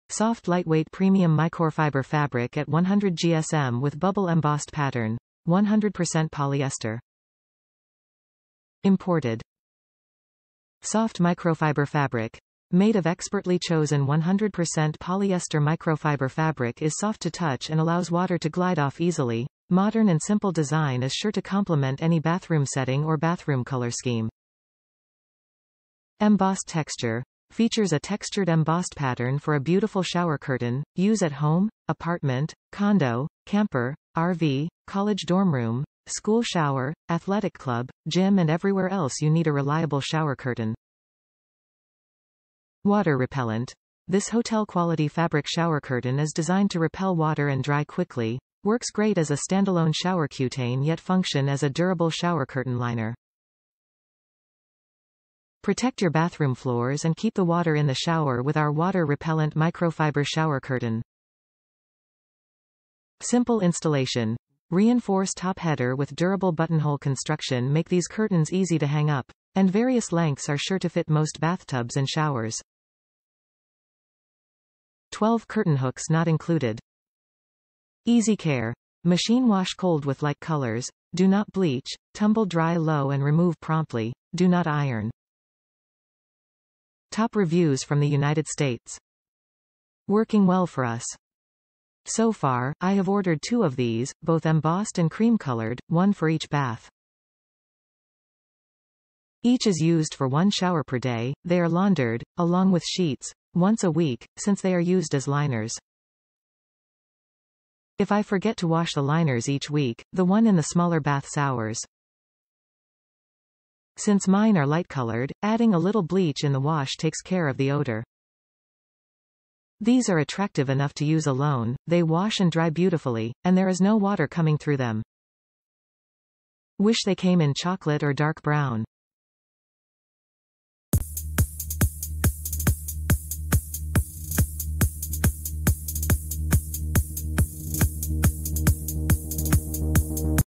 CAROMIO Shower Curtain, Water Repellent Soft Microfiber Fabric Shower Curtain or Liner for Bathroom
Soft Lightweight Premium Micorfiber Fabric at 100gsm With Bubble Embossed Pattern., 100% Polyester
Imported
Soft Microfiber Fabric: Made of expertly chosen 100% polyester microfiber fabric is soft to touch and allows water to glide off easily, modern and simple design is sure to compliment any bathroom setting or bathroom color scheme.
Embossed Texture: Features a textured embossed pattern for a beautiful shower curtain,;use at home, apartment, condo, camper, RV, college dorm room, school shower, athletic club, gym and everywhere else you need a reliable shower curtain.
Water Repellent: This hotel quality fabric shower curtain is designed to repel water and dry quickly, works great as a standalone shower cutain yet function as a durable shower curtain liner. Protect your bathroom floors and keep the water in the shower with our Water Repellent Microfiber Shower Curtain.
Simple Installation: Reinforce top header with durable buttonhole construction make these curtains easy to hang up, and various lengths are sure to fit most bathtubs and showers. (12 curtain hooks not included)
Easy Care: Machine wash cold with like colors; Do not bleach; Tumble dry low and remove promptly; Do not iron.

Working Well for Us
So far, I have ordered two of these, both embossed and cream colored, one for each bath. Each is used for one shower per day, they are laundered (along with sheets) once a week, since they are used as liners. If I forget to wash the liners each week, the one in the smaller bath sours. Since mine are light colored, adding a little bleach in the wash takes care of the odor. These are attractive enough to use alone, they wash and dry beautifully, and there is no water coming through them. Wish they came in chocolate or dark brown!